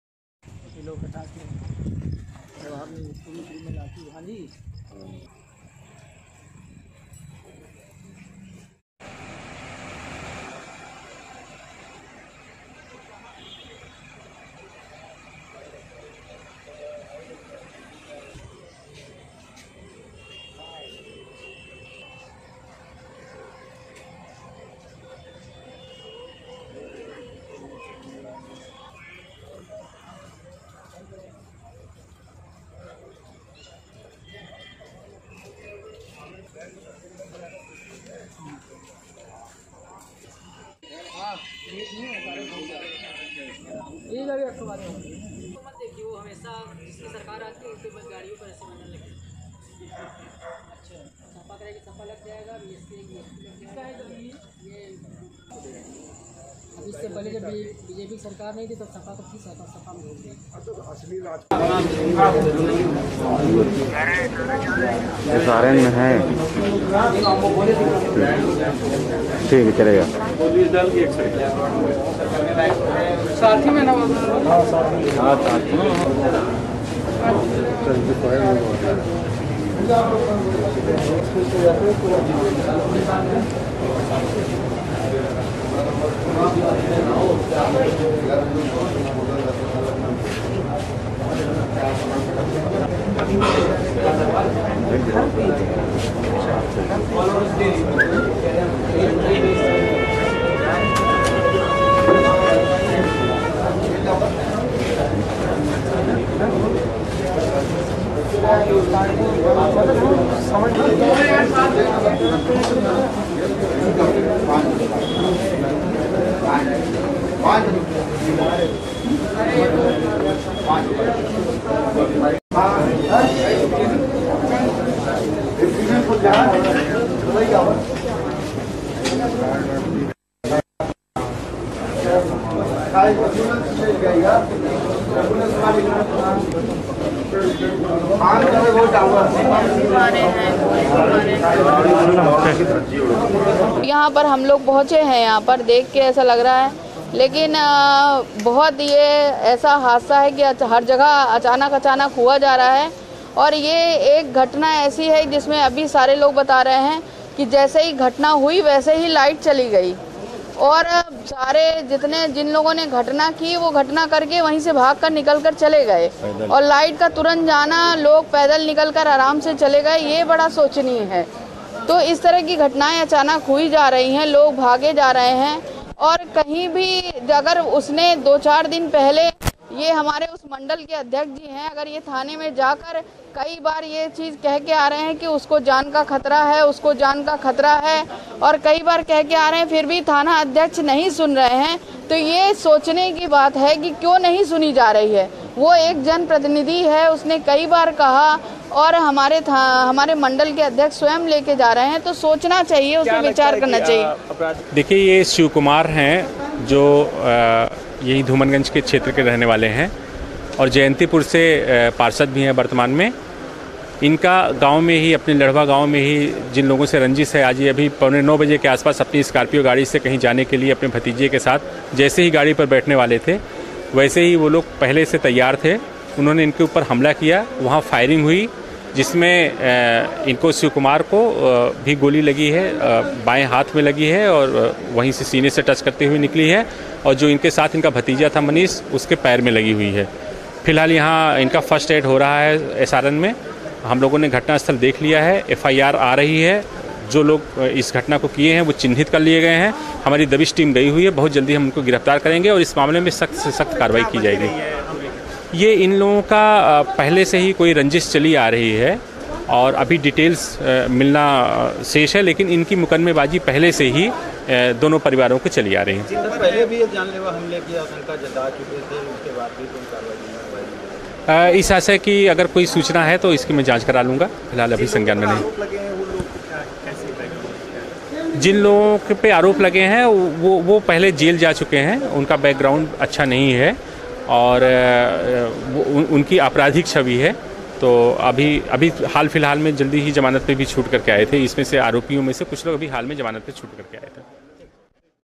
प्रयागराज मे बीजेपी नेता पर ताबड़तोड़ फायरिंग और बमबाजी
बीजेपी नेता पर हमला, पुलिस पर बरसीं मेयर अभिलाषा

बीजेपी नेता पर हमले के मामले में प्रयागराज की मेयर अभिलाषा गुप्ता ने पुलिस प्रशासन पर सवाल खड़े किए हैं। मेयर ने शुक्रवार रात स्वरूपरानी अस्पताल जाकर पार्षद शिव कुमार भारतीय का हालचाल जाना। इस दौरान वह पुलिस अधिकारियों पर जमकर बरसीं। उन्होंने पत्रकारों से कहा कि आजकल हर जगह ऐसी घटनाएं सामने आ रही हैं। मेयर ने कहा कि शुक्रवार रात में हमले के फौरन बाद लाइट चली गई और हमलावर मौके से पैदल ही बड़े आराम से चले गए। उन्होंने कहा कि अगर नेता थानाध्यक्ष को बताते हैं कि उनकी जान को खतरा है फिर भी उनकी बात नहीं सुनी जाती है। यह सोचने वाली बात है कि आखिर उनकी बात क्यों नहीं सुनी जा रही है। उन्होंने कहा कि पार्टी के मंडल अध्यक्ष खुद पुलिस के पास गए थे और पार्षद पर हमले की आशंका जाहिर की थी, इसके बाद भी पुलिस ने कोई कार्रवाई नहीं की।

उधर, पुलिस के एक वरिष्ठ अधिकारी ने बताया कि धूमनगंज के रहने वाले शिव कुमार जयंतीपुर से पार्षद हैं। गांव में इनका विवाद था और शुक्रवार रात में पौने 9 बजे यह और इनका भतीजा गाड़ी में बैठने वाले थे तभी पहले से तैयार बैठे हमलावरों ने फायरिंग शुरू कर दी। शिवकुमार को बाएं हाथ में गोली लगी है और सीने से छूती हुई निकली है। भतीजा भी घायल है। पुलिस के मुताबिक दो परिवारों की आपसी रंजिश काफी समय से चल रही थी। हमलावरों की पहचान कर ली गई है और पुलिस की तरफ से छापेमारी शुरू हो गई है। पुलिस ने दावा किया है कि जल्द ही हमलावरों को गिरफ्तार कर लिया जाएगा। आरोपियों की छवि अच्छी नहीं है। वे हाल ही में जमानत पर छूट कर आए हैं।